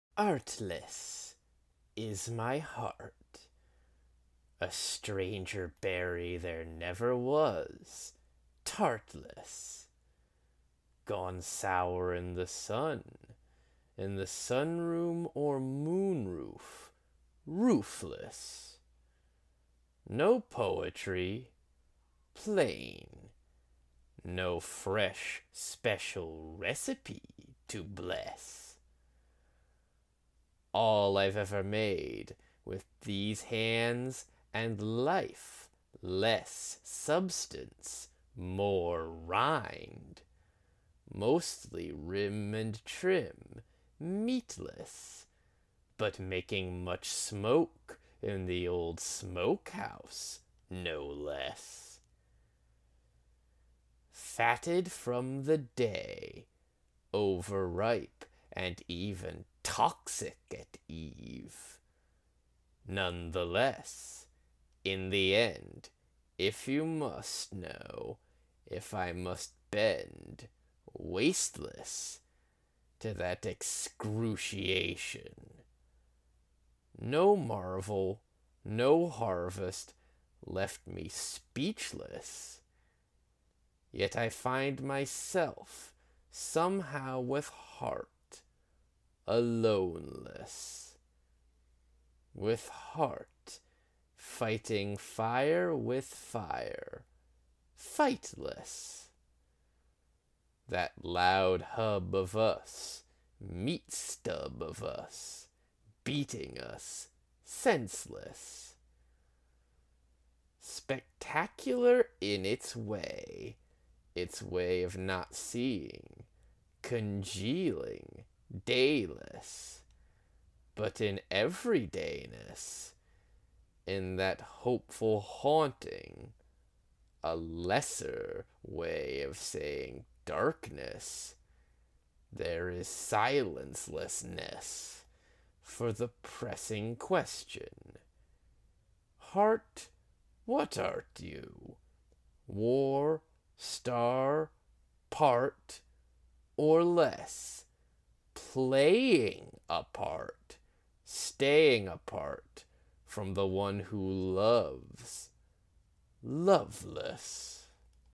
TripleHate Reads: Artless (Brenda Shaughnessy)
Today's selection uses some most intriguing distortion of the typical format (see pic) to convey the frustration of a poet to turn a painful experience into art, a frustration all creators can share when one reaches into one's own well of experience for inspiration, only for the bucket to come up empty...